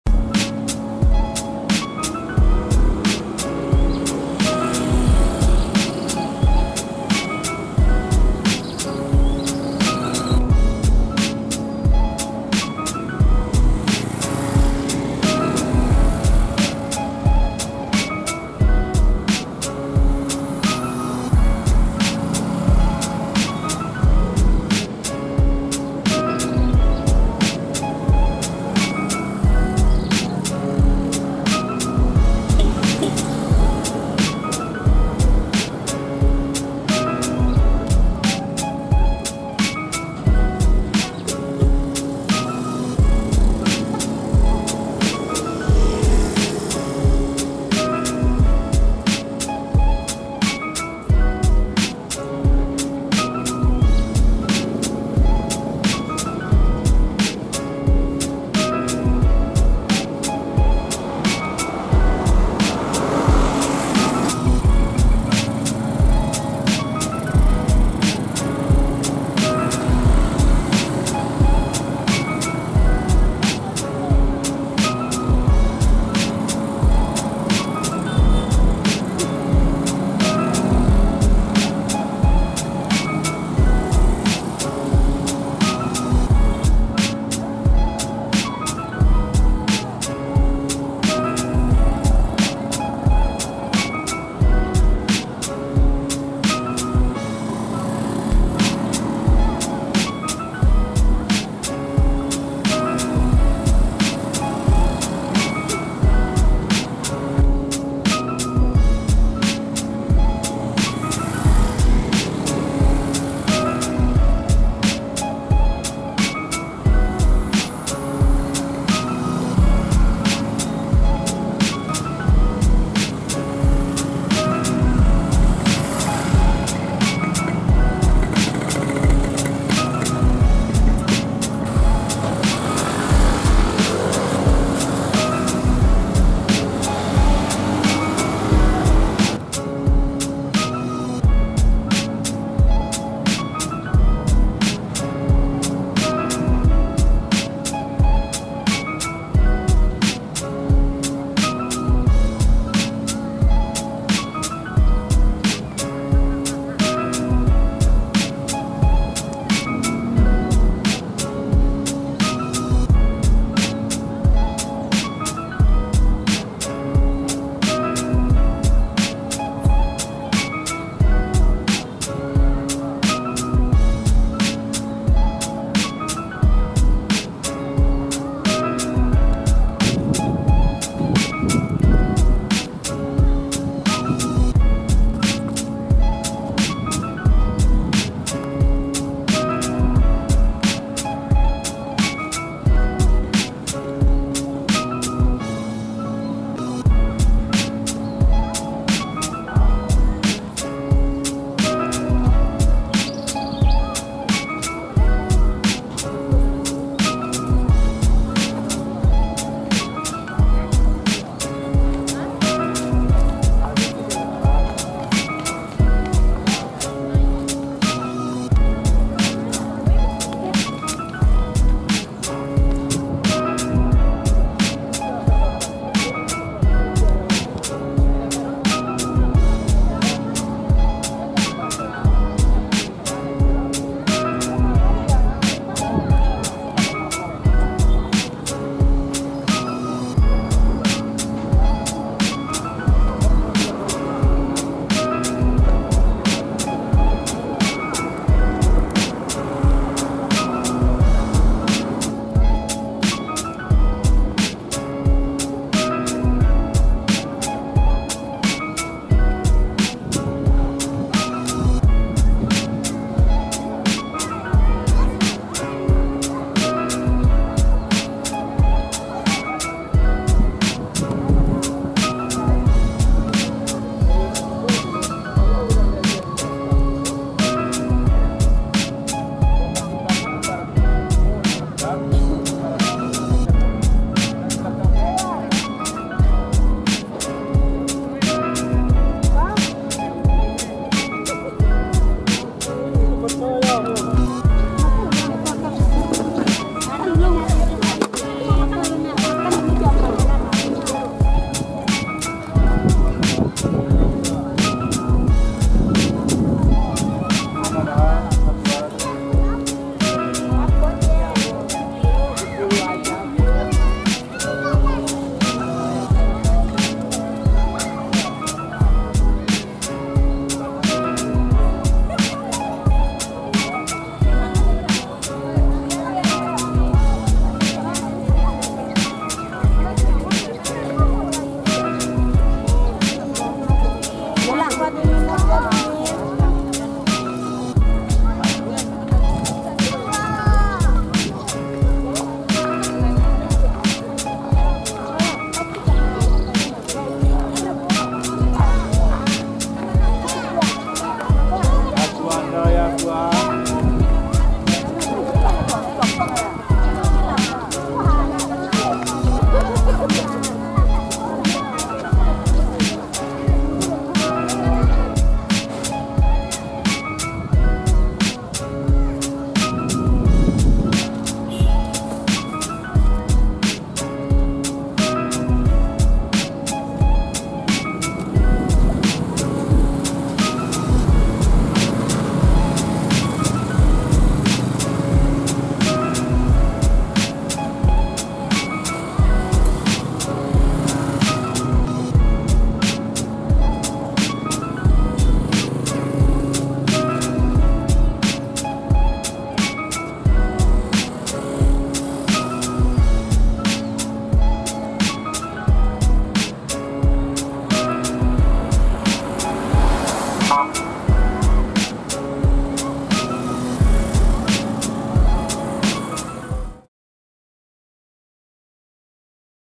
The Charm of Jam Gadang Bukit Tinggi//Pesona Jam Gadang Bukit Tinggi
Trips to Bukit Tinggi must stop at Jam Gadang. His charm is spectacular. Cool city close to Mount Marapi and Gunung Singgalang. Jam Gadang is one of these City Icons//Berwisata ke Bukit Tinggi pasti mampir ke Jam Gadang.  Pesonanya sangat spektakuler. Kota sejuk dekat dengan gunung Marapi dan Gunung Singgalang. Jam Gadang merupakan salah satu Ikon Kota Ini